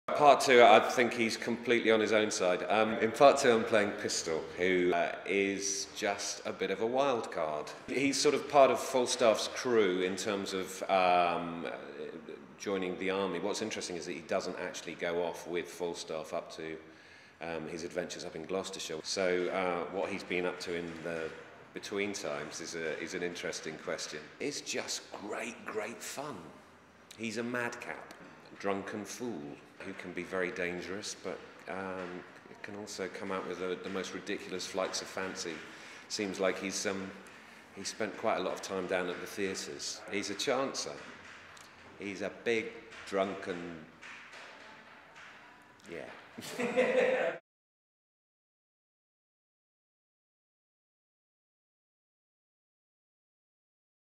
Meet the actors - Antony Byrne | Henry IV | Royal Shakespeare Company
Antony Byrne tells us more about the character of Pistol in Henry IV Part II.

Henry IV Parts I & II plays at London's Barbican from 29 November - 24 January.